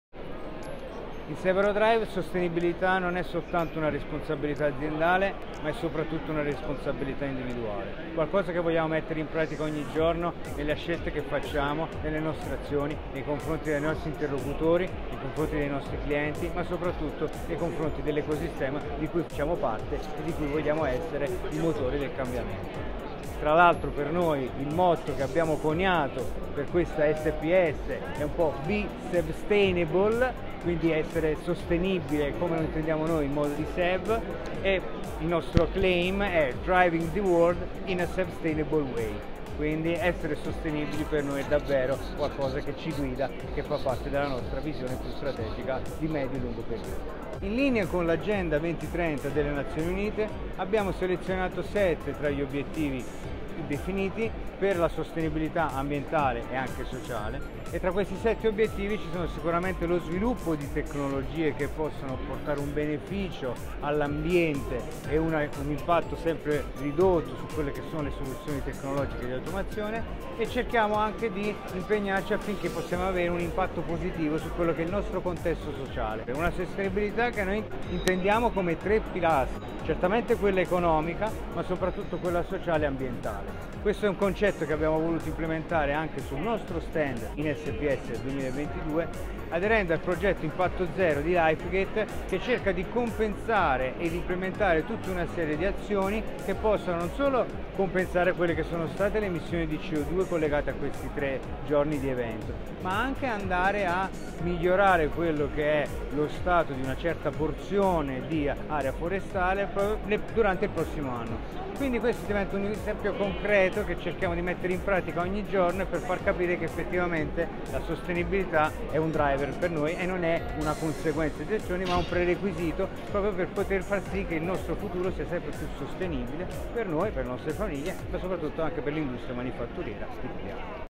SPS Italia 2022: la sostenibilità di SEW-Eurodrive
A SPS Italia 2022 Giosuè Cavallaro, marketing manager, racconta il progetto di sostenibilità di SEW-Eurodrive Italia